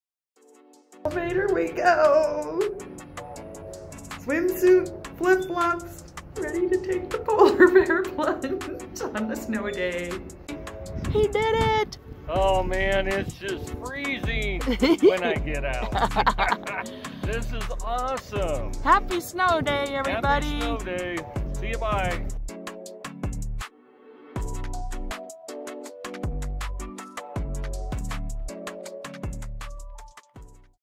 Loki approved snow day
While Loki's mastering the art of snow zoomies, John's decided to take a dip in the pool, channeling his inner polar bear. Ah, the joys of contrasting preferences! 🐻❄️💦 wish you were here!